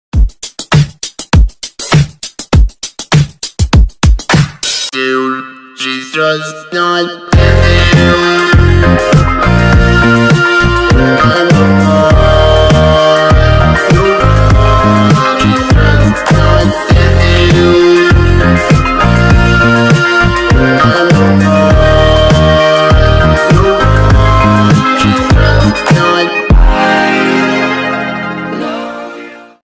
Dude She’s Just Not Into You Meme//Extended// Not a Vent
Guys I never makes vents and this isn’t one either :)
Hello! Hi! How ya doin 😏. This meme didn’t take as much effort as I thought it would, my phone recently killed ibis paint x so now I have to use 2 devices in order to make a meme ;( but yeah I hope you enjoy finally it’s done ^^